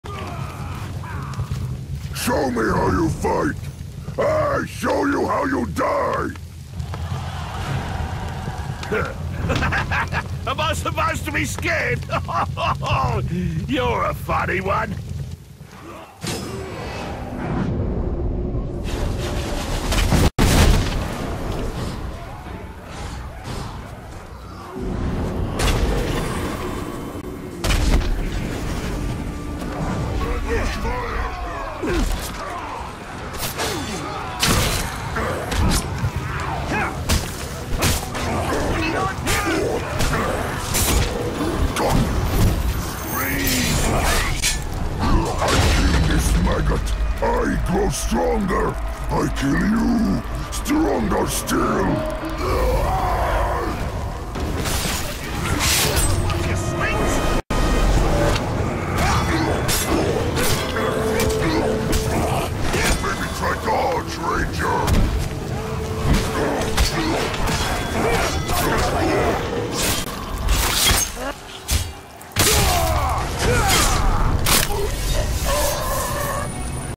Ar Janek wants to be Stronger
Ar-Janek and Pug started a duel which I interrupted and Ended.